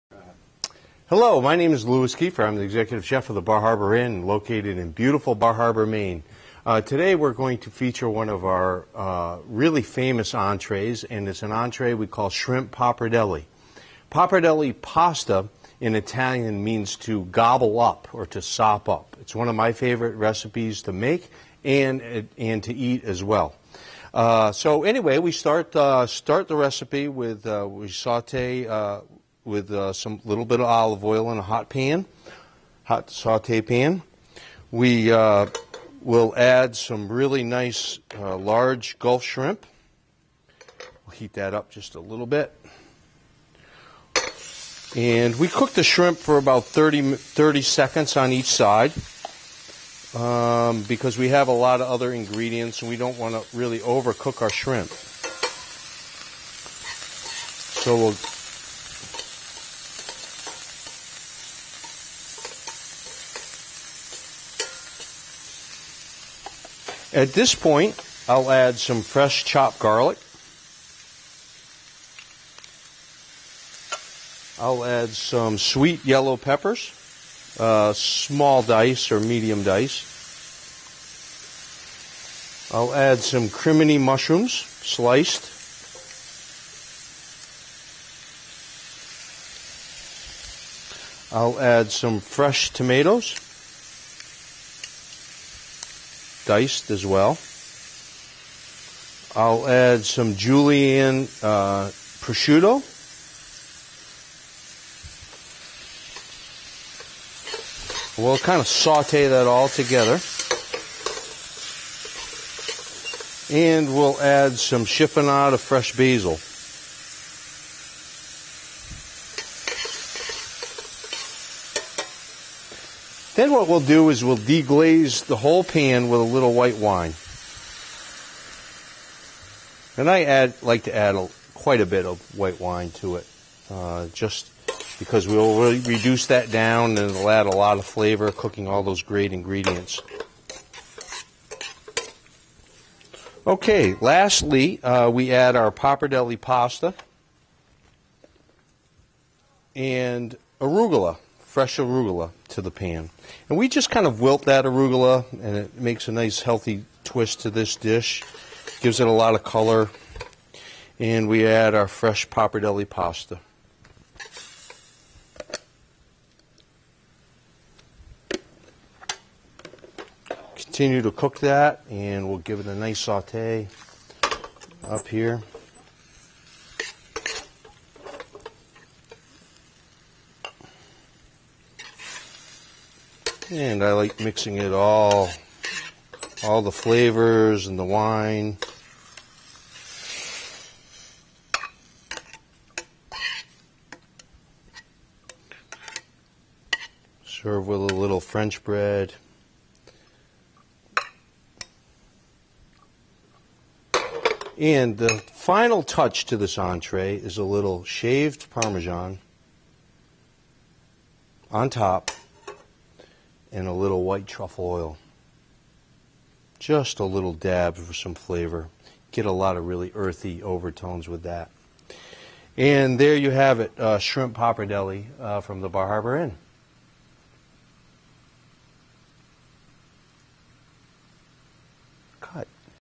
Bar Harbor Inn Chef Kiefer with a Culinary Demonstration of Shrimp Pappardelle
Chef Kiefer gives us a culinary demonstration of one of his favorite
dishes shrimp pappardelle